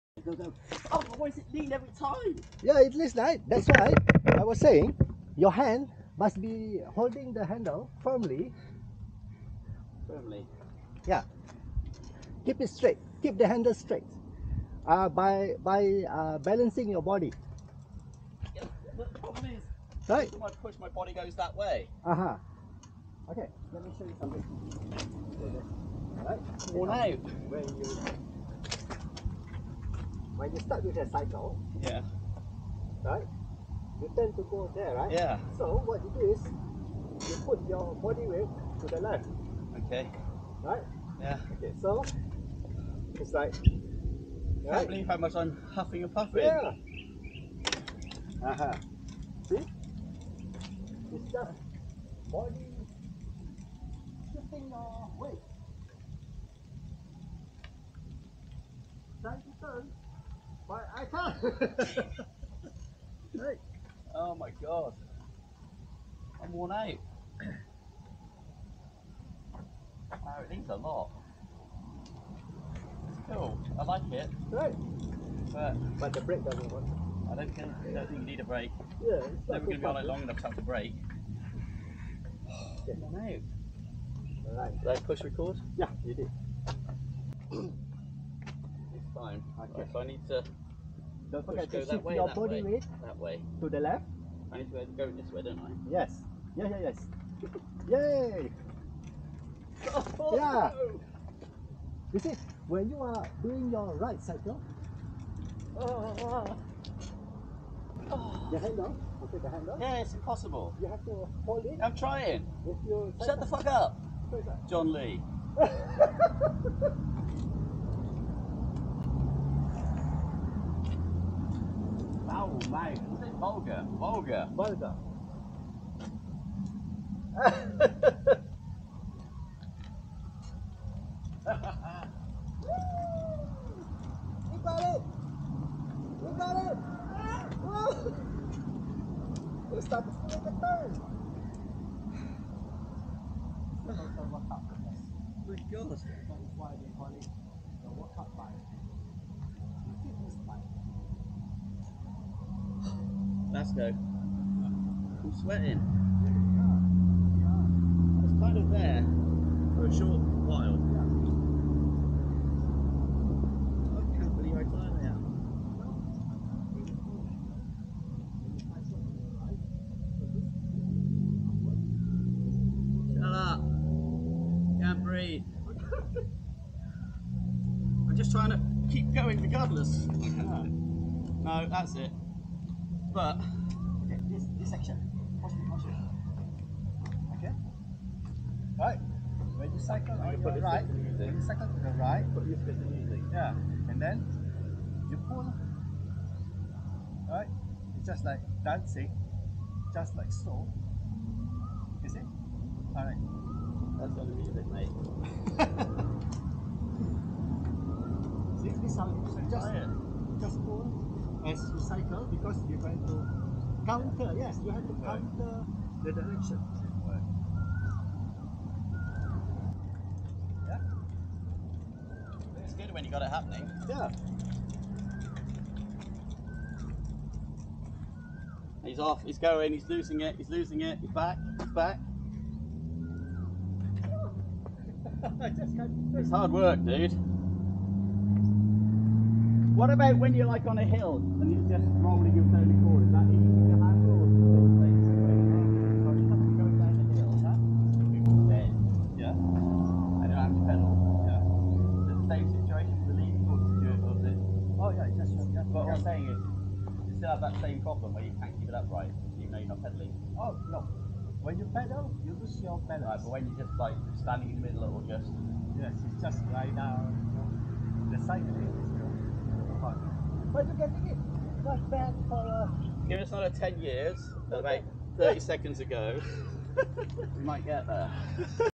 Guiding Silverfox the tutorial on how to ride / cycle the Bowqi half bike
As surprised as I was that he would put on a Malay traditional baju Melayu costume upon my request, I am also astounded that he was able to do this feat too! 😳 So for your viewing pleasure, here's me, guiding Silverfox the tutorial on how to ride / cycle the Bowqi half bike.